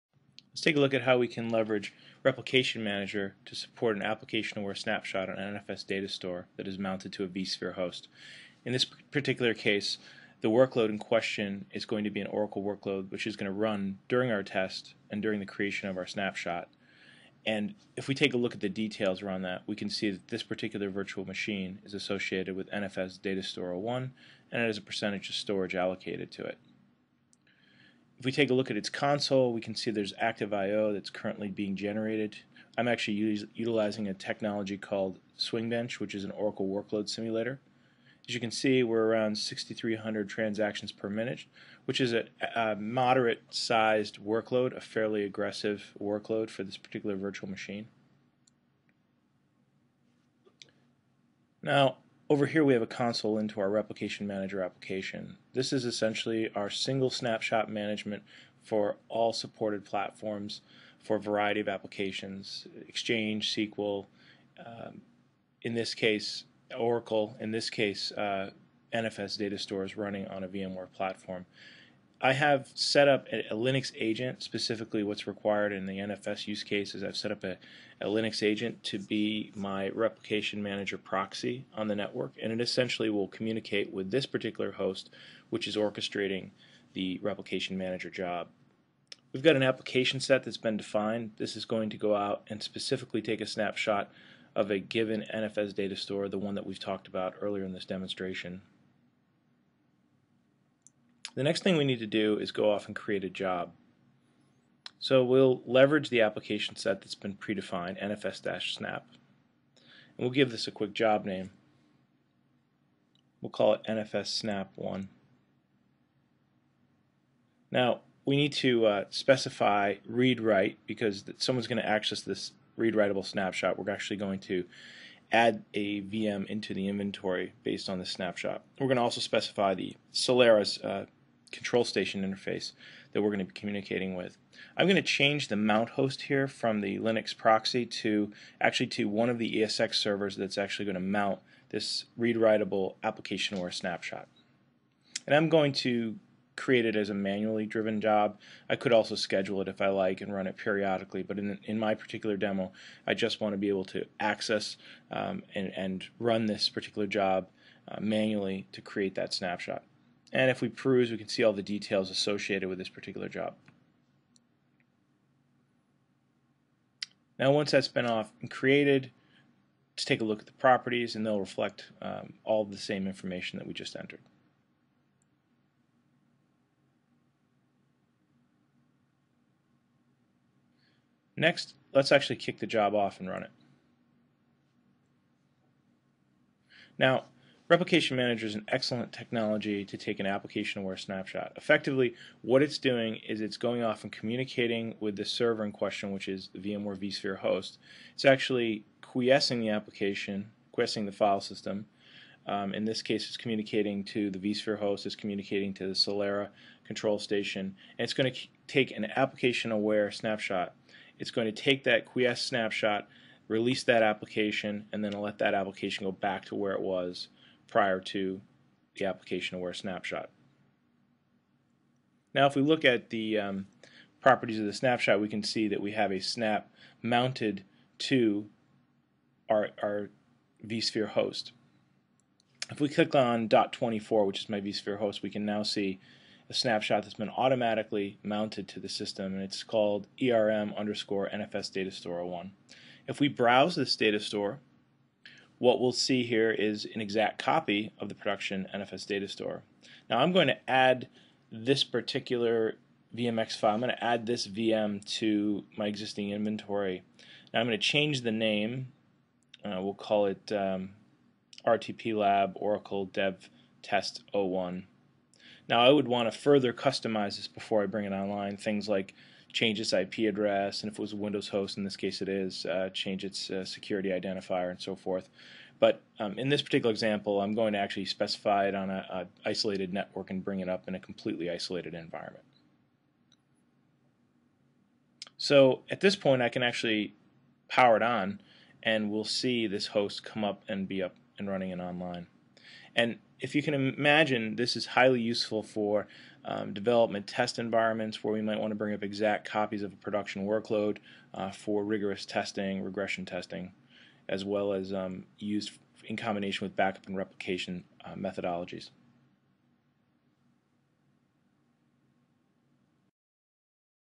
EMC Replication Manager on vSphere with Oracle
This is a quick demonstration done for a customer who needed a simple way to create test/dev replicas of their Oracle VMs on NFS datastores.